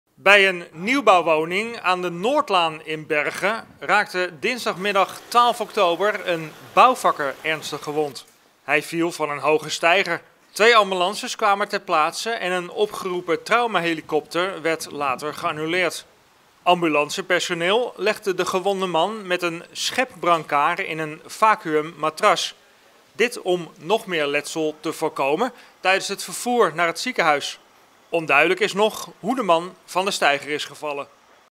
Bouwvakker gewond na val van steiger
BERGEN - Een bouwvakker raakte dinsdagmiddag 12 oktober 2021 bij een woning aan de Noordlaan in Bergen ernstig gewond bij een val van een steiger.